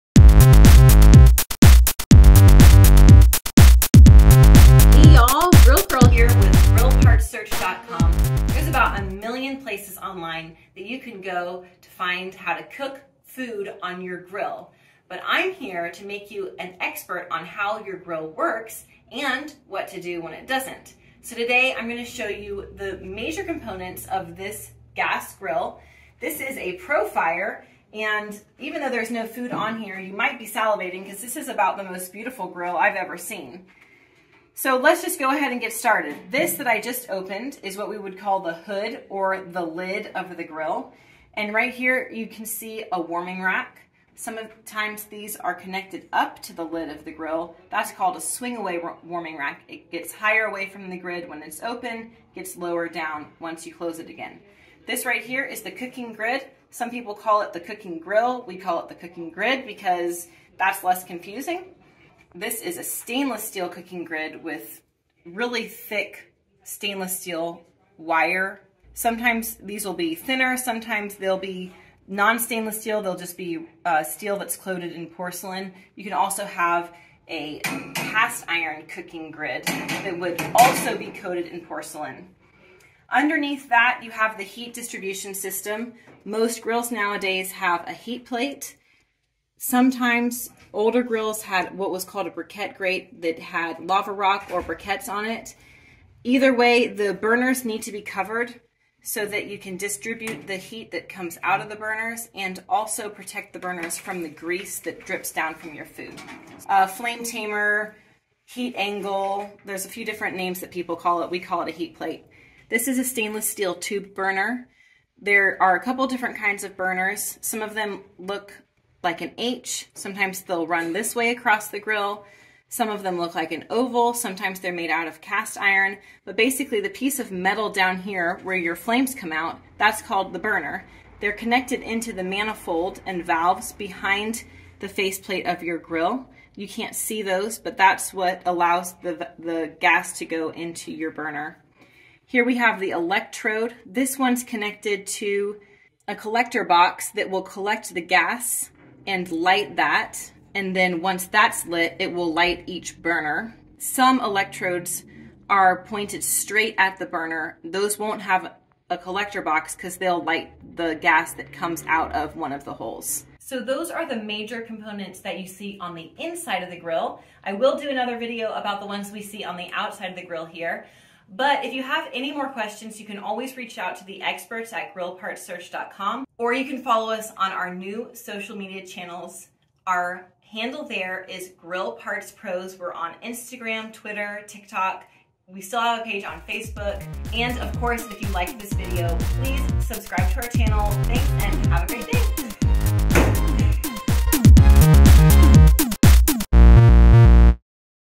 What's In a Gas Grill? Major Parts Inside a Gas Grill Explained
Think there might something *off* in your gas grill, but don't know where to start? Wondering what the main parts of a gas grill are called? Grill Girl explains the different parts you see inside a gas grill. From burners and cooking grids to heat distribution and ignitor components, check out this video to find out more about the terms used for parts of your grill! Once you know what might be wrong, you can search our website to find a replacement or give us a call for troubleshooting. We're the experts in gas grill repair and here to help you get back to grillin'!